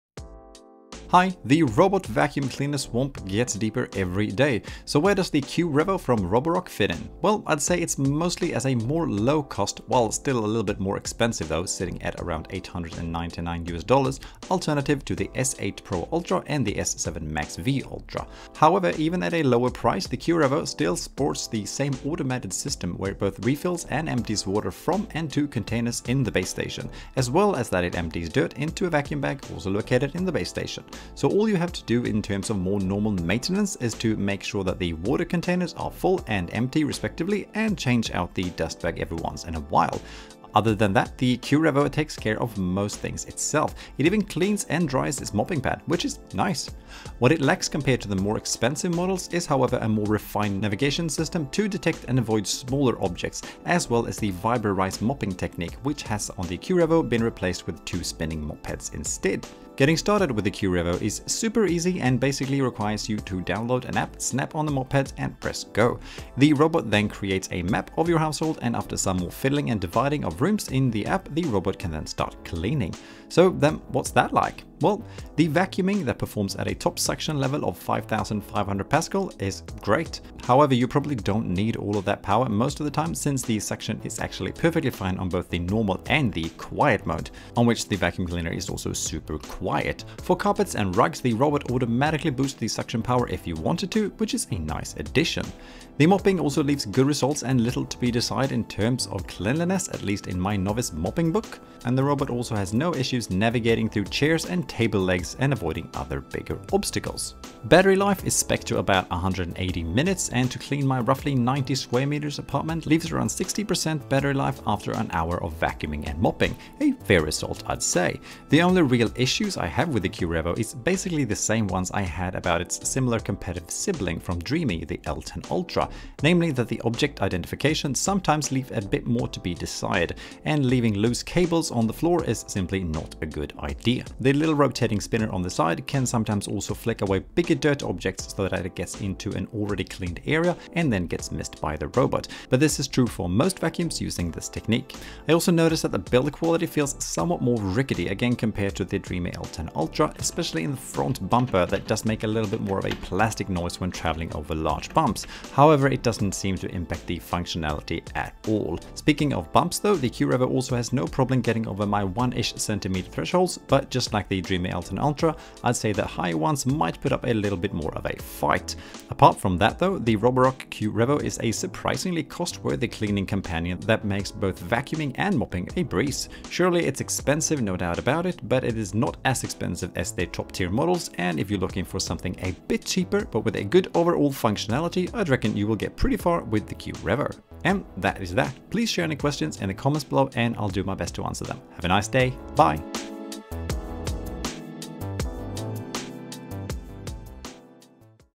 Roborock Q Revo | A Quick Review
This is my short review of the Roborock Q Revo robot vacuum cleaner. A somewhat more cheap, but still great, alternative to the S8 Pro Ultra and the S7 MaxV Ultra.